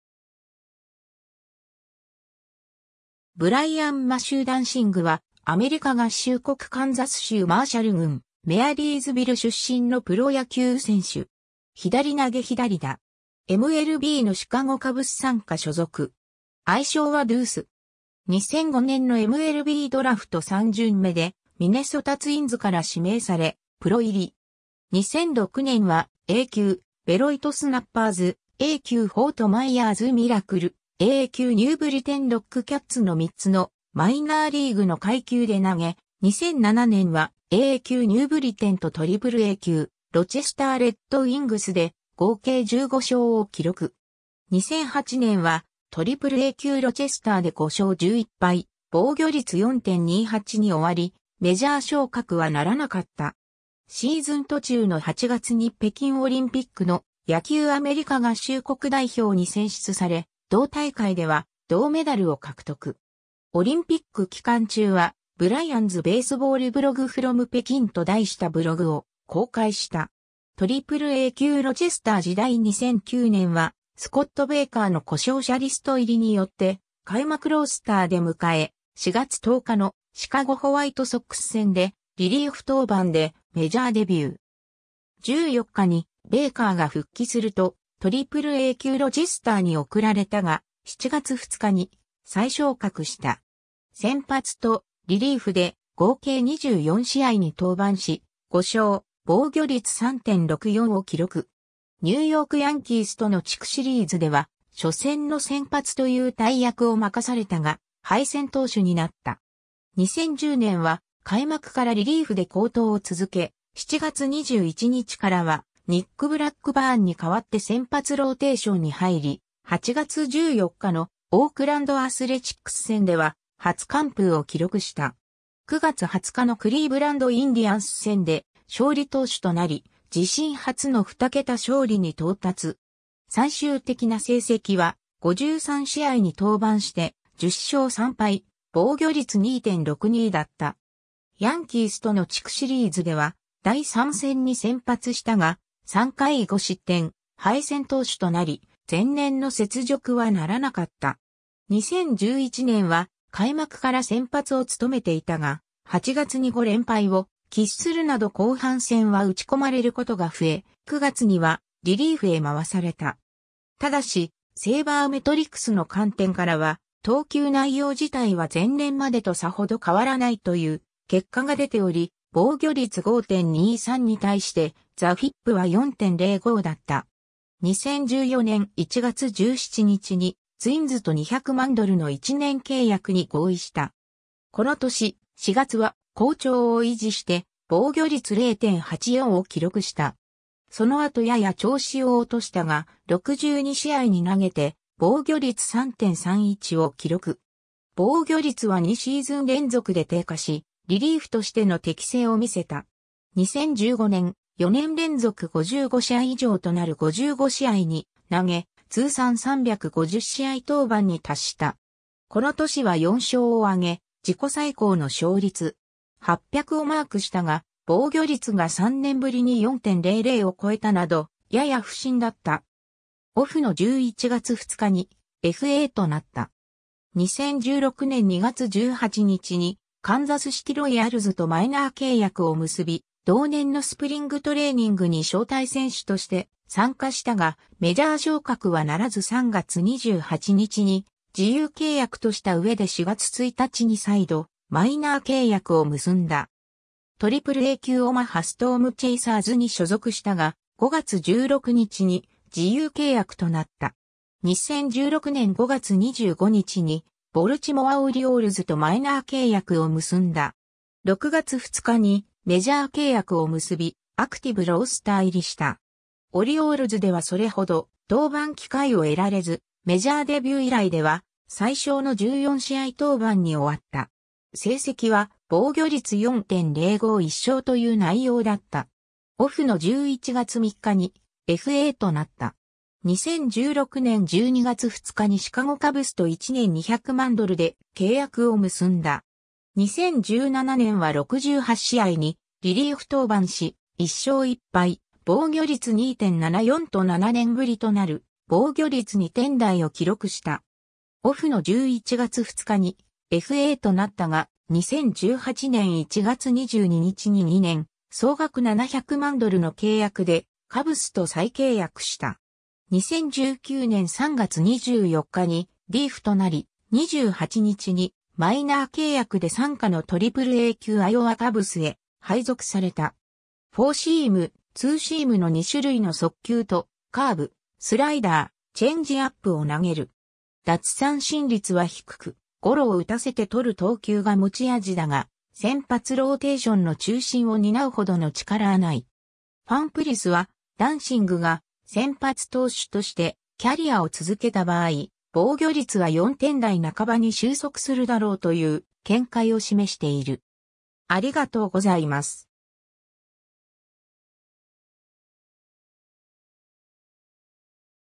ブライアン・ダンシング
カンザスシティ・ロイヤルズ傘下の選手
ブライアン・マシュー・ダンシング（Brian Matthew Duensing, 1983年2月22日 - ）は、アメリカ合衆国カンザス州マーシャル郡メアリーズビル出身のプロ野球選手（投手）。
左投左打。
MLBのシカゴ・カブス傘下所属。
愛称はドゥース（Duece）。
 2005年のMLBドラフト3巡目（全体84位）でミネソタ・ツインズから指名され、プロ入り。
 2006年はA級ベロイト・スナッパーズ、A+級フォートマイヤーズ・ミラクル、AA級ニューブリテン・ロックキャッツの3つのマイナーリーグの階級で投げ、2007年はAA級ニューブリテンとAAA級ロチェスター・レッドウイングスで合計15勝を記録。
 2008年はAAA級ロチェスターで5勝11敗、防御率4.28に終わり、メジャー昇格はならなかった。
シーズン途中の8月に北京オリンピックの野球アメリカ合衆国代表に選出され、同大会では銅メダルを獲得。
オリンピック期間中は、「Brian's Baseball Blog from Beijing」と題したブログを公開した。
 AAA級ロチェスター時代(2008年) 2009年はスコット・ベイカーの故障者リスト入りによって開幕ロースターで迎え、4月10日のシカゴ・ホワイトソックス戦でリリーフ登板でメジャーデビュー。
14日にベイカーが復帰するとAAA級ロチェスターに送られたが、7月2日に再昇格した。
先発とリリーフで合計24試合に登板し、5勝・防御率3.64を記録。
ニューヨーク・ヤンキースとの地区シリーズでは初戦の先発という大役を任されたが、敗戦投手になった。
 2010年は開幕からリリーフで好投を続け、7月21日からはニック・ブラックバーンに代わって先発ローテーションに入り、8月14日のオークランド・アスレチックス戦では初完封を記録した。
9月20日のクリーブランド・インディアンス戦で勝利投手となり、自身初の二桁勝利に到達。
最終的な成績は、53試合（先発は13試合）に登板して、10勝3敗、防御率2.62だった。
ヤンキースとの地区シリーズでは第3戦に先発したが、3回5失点敗戦投手となり、前年の雪辱はならなかった。
 2011年は開幕から先発を務めていたが、8月に5連敗を喫するなど後半戦は打ちこまれることが増え、9月にはリリーフへ回された。
ただし、セイバーメトリクスの観点からは、投球内容自体は前年までとさほど変わらないという結果が出ており、防御率5.23に対して、xFIPは4.05だった（前年は3.95、キャリア平均は4.17）。
 2014年1月17日にツインズと200万ドルの1年契約に合意した。
この年、4月は好調を維持して防御率0.84を記録した。
その後やや調子を落としたが、62試合に投げて防御率3.31を記録。
防御率は2シーズン連続で低下し、リリーフとしての適性を見せた。
 2015年、4年連続55試合以上となる55試合に投げ、通算350試合登板に達した。
この年は4勝（1敗）を挙げ、自己最高の勝率.800をマークしたが、防御率が3年ぶりに4.00を超えた（4.25）など、やや不振だった。
 2016年2月18日にカンザスシティ・ロイヤルズとマイナー契約を結び、同年のスプリングトレーニングに招待選手として参加したが、メジャー昇格はならず3月28日に自由契約としたうえで4月1日に再度マイナー契約を結んだ。
AAA級オマハ・ストームチェイサーズに所属したが、5月16日に自由契約となった。
 2016年5月25日にボルチモア・オリオールズとマイナー契約を結んだ。
6月2日にメジャー契約を結び、アクティブ・ロースター入りした。
オリオールズではそれほど登板機会を得られず、メジャーデビュー以来では最小の14試合登板に終わった。
成績は、防御率4.05・1勝という内容だった。
 2016年12月2日にシカゴ・カブスと1年200万ドルで契約を結んだ。
 2017年は68試合にリリーフ登板し、1勝1敗・防御率2.74と7年ぶり（リリーフ専念後では初）となる防御率2点台を記録した...